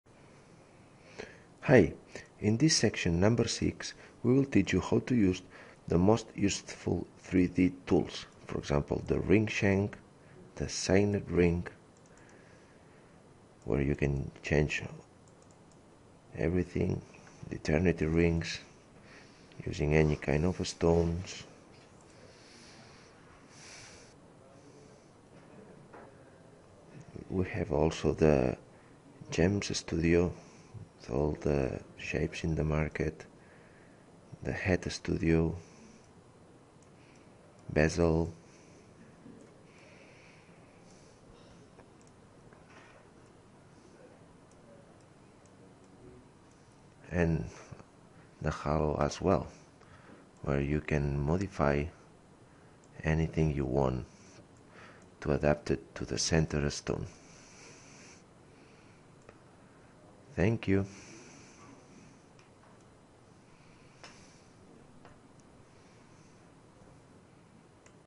RhinoGold 6 3D Tools
A quick look at some of the topics that we will show you during training here in Barcelona or Lafayette.